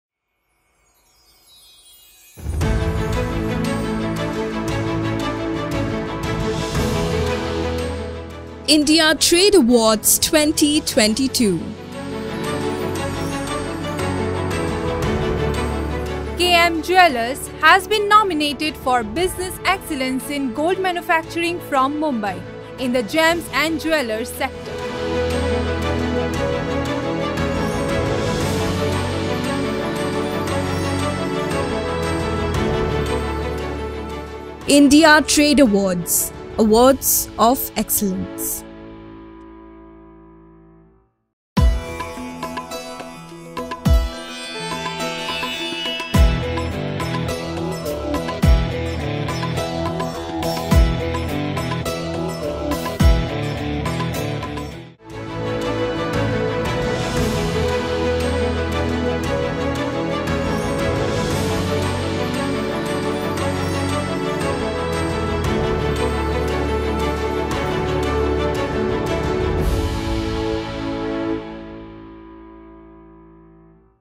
KM Jewellers has been nominated for India Trade Awards 2022.
The India Trade Awards is a great opportunity for entrepreneurs and businesses to be recognized, showcase, and celebrate hard-earned success.

India Trade Awards provide a diverse range of business sectors to highlight success in the business world.